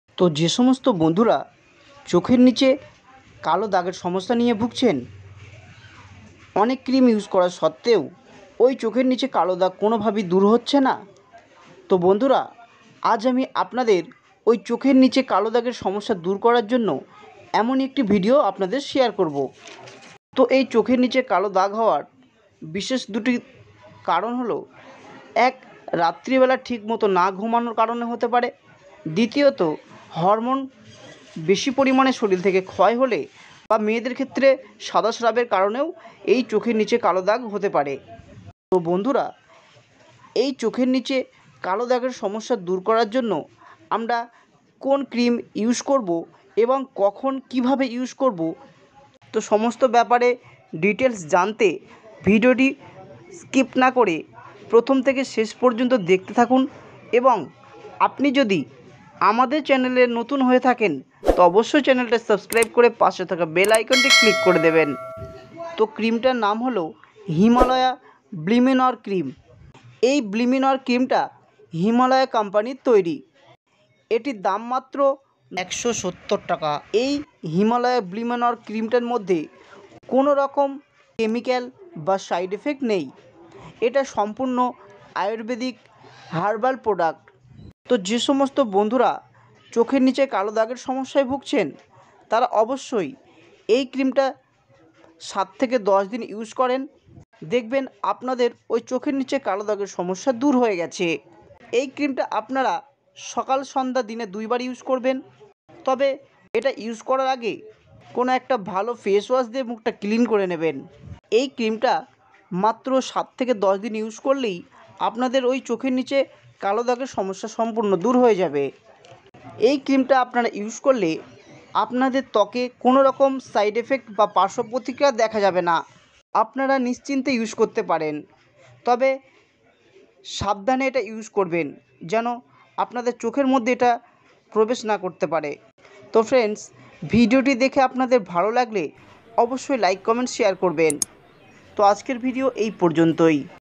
চোখের নিচে কালো দাগ দূর করার ক্রিম।। Himalaya Bleminor Anti Blemish Cream in Bangla Review
চোখের নিচে কালো দাগ দূর করার সবচেয়ে ভালো ক্রিম
এই Cream tar  দাম মাত্র 170 টাকা
এটি সম্পূর্ণ আয়ুর্বেদিক প্রোডাক্ট
এই cream tar কোনো সাইডএফেক্ট বা পার্শ্ব প্রতিক্রিয়া নেই